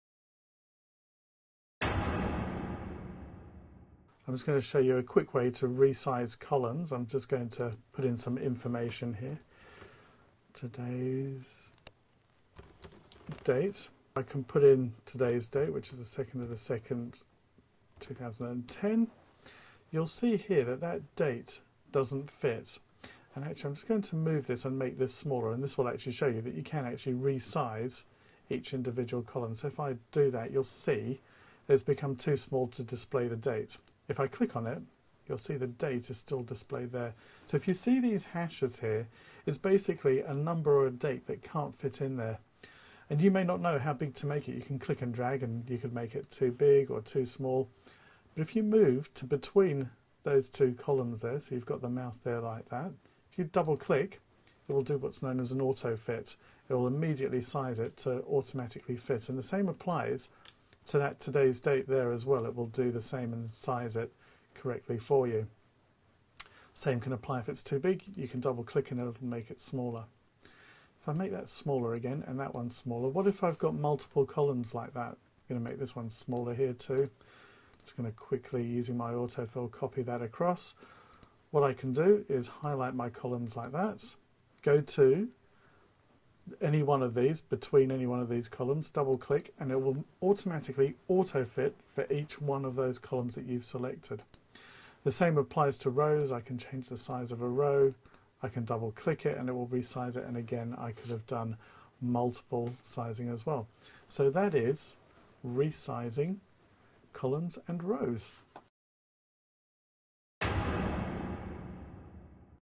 Resizing Columns and Rows in Excel
Here are quick ways to resize columns and rows in Excel. No menus just clicks with the mouse. Presented by Gary Schwartz